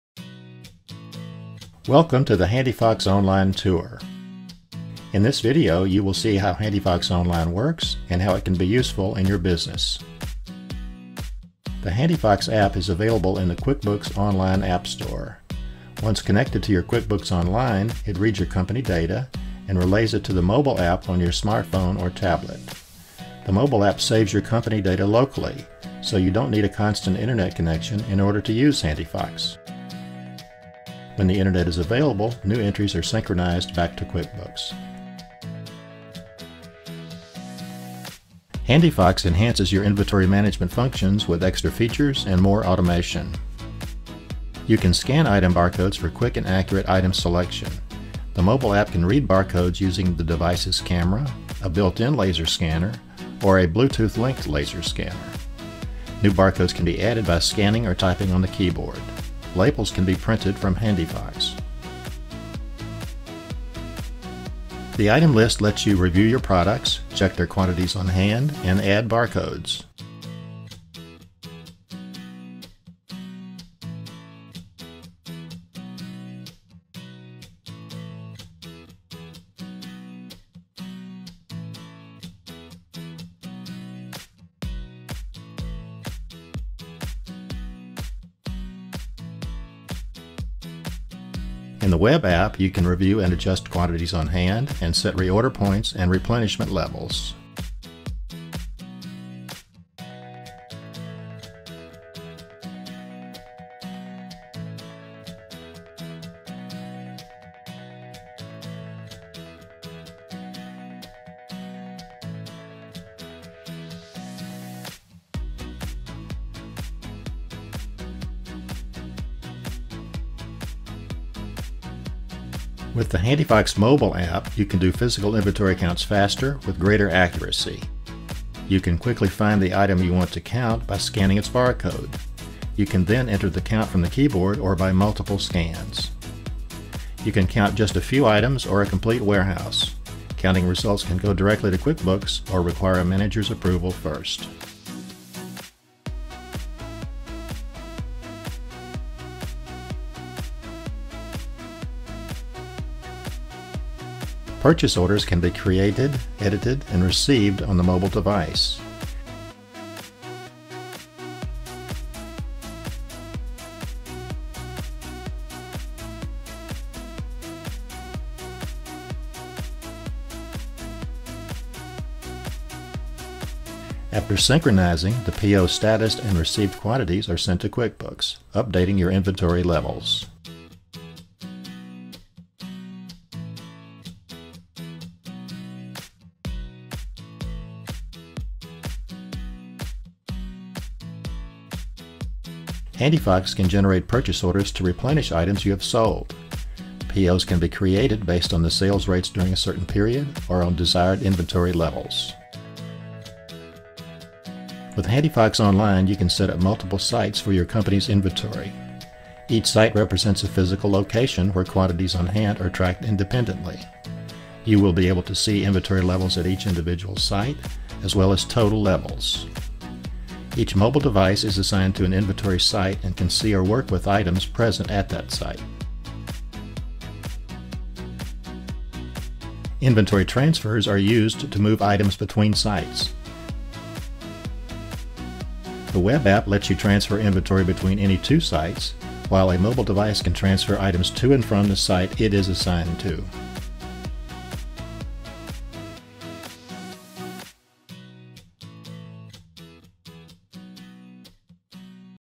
HandiFox Online Start Overview
This quick overview will walk you through the feature set of the cloud-based inventory management system HandiFox Online START version -  powerful inventory control software integrated with QuickBooks Online